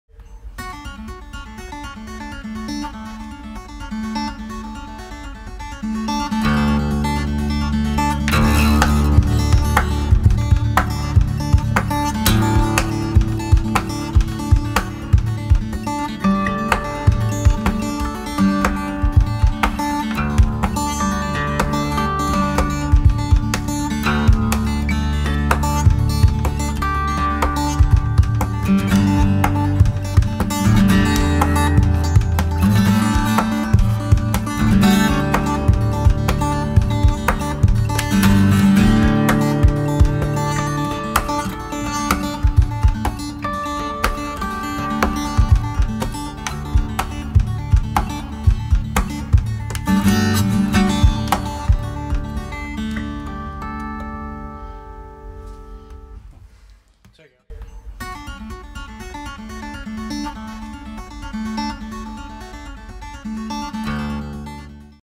Welcome to my channel - Juan P. Ordoñez
Hope you guys like the fingerstyle acoustic guitar videos, music production tips, lead guitar techniques, drum, bass...More stuff coming soon.

Thanks!